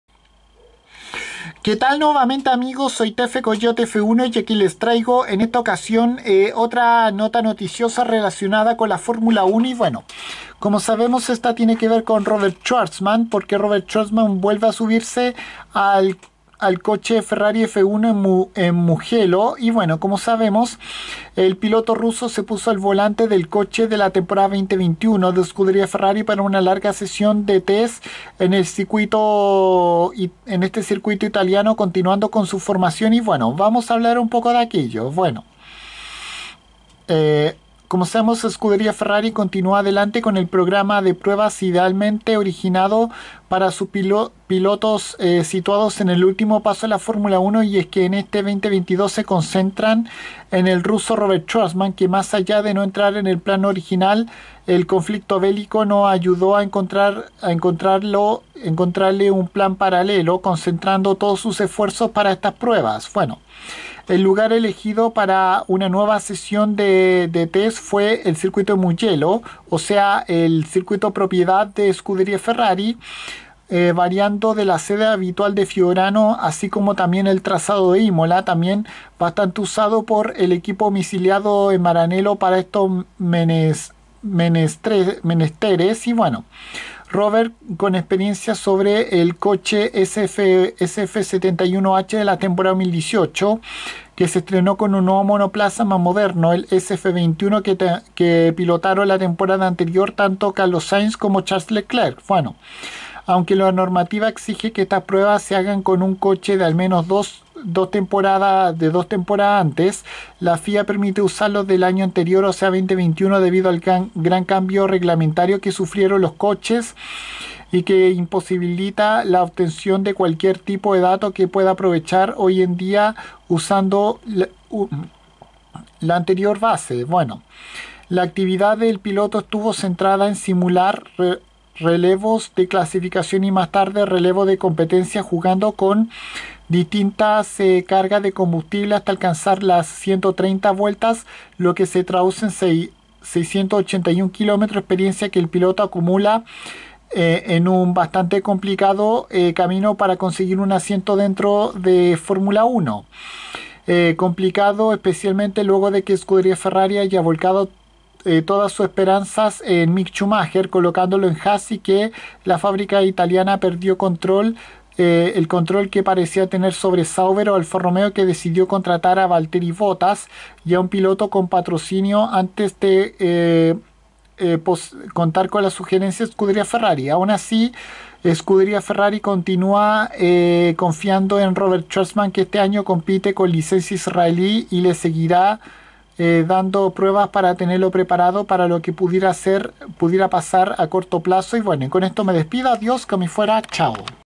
Robert Shwartzman volvió a estar en un coche F1 de Ferrari en Mugello
El joven piloto ruso estuvo manejando el coche SF21 de la anterior Temporada 2021 de Fórmula 1 de Scuderia Ferrari para continuar con su formación y que ahora compite con licencia israelí